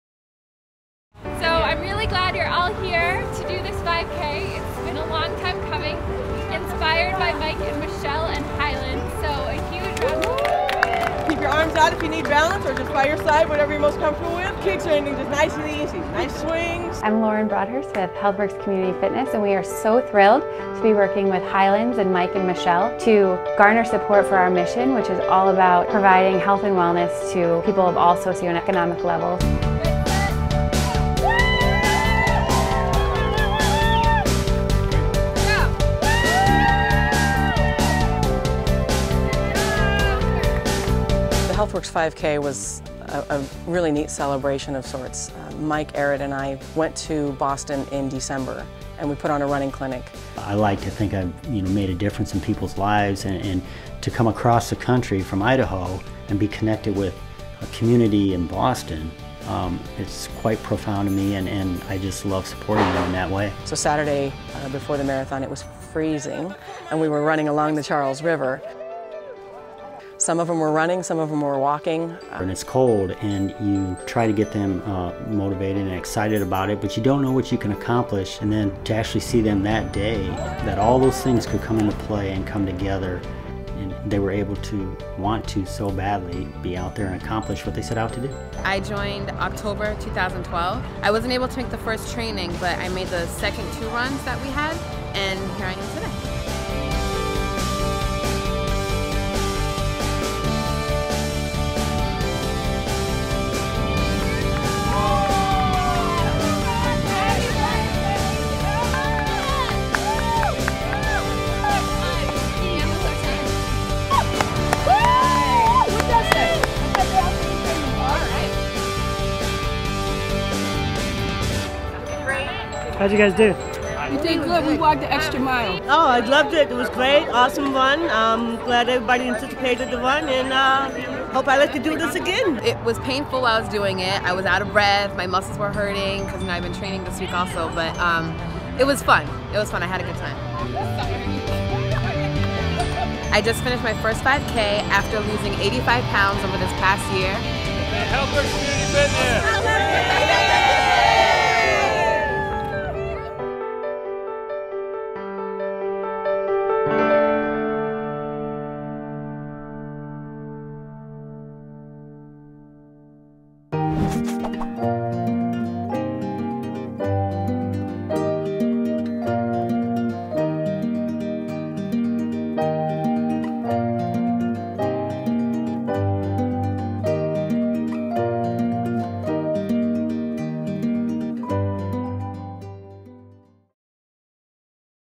Healthworks Community Fitness 5K Run/Walk in Boston
Healthworks Community Fitness held an informal 5K Run/Walk along the Charles River in Boston, which had many participants running their first ever 5K.